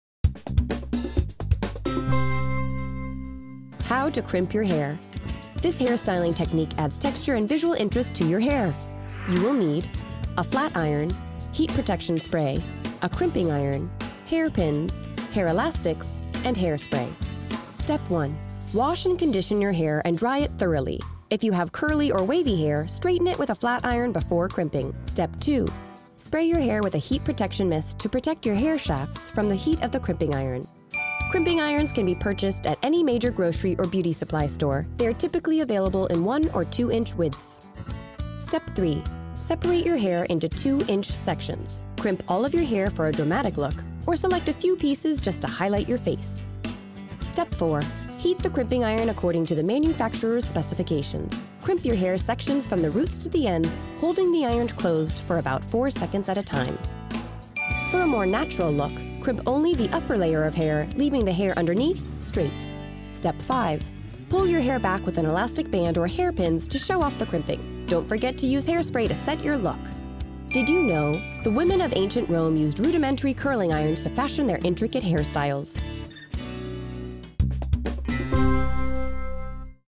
How to Crimp Your Hair
This hairstyling technique adds texture and visual interest to your hair.

Step 1: Wash, condition, and dry hair
Wash and condition your hair and dry it thoroughly. If you have curly or wavy hair, straighten it with a flat iron before crimping.

Step 2: Protect hair
Spray your hair with a heat protection mist to protect your hair shafts from the heat of the crimping iron.

Tip
Crimping irons can be purchased at any major grocery or beauty supply store. They are typically available in 1- or 2-inch widths.

Step 3: Separate hair
Separate your hair into 2-inch sections. Crimp all of your hair for a dramatic look or select a few pieces just to highlight your face.

Step 4: Use crimping iron
Heat the crimping iron according to the manufacturer's specifications. Crimp your hair sections from the roots to the ends, holding the iron closed for about 4 seconds at a time.

Tip
For a more natural look, crimp only the upper layer of hair, leaving the hair underneath straight.

Step 5: Set hair
Pull your hair back with an elastic band or hair pins to show off the crimping. Don’t forget to use hairspray to set your look.

The women of Ancient Rome used rudimentary curling irons to fashion their intricate hairstyles.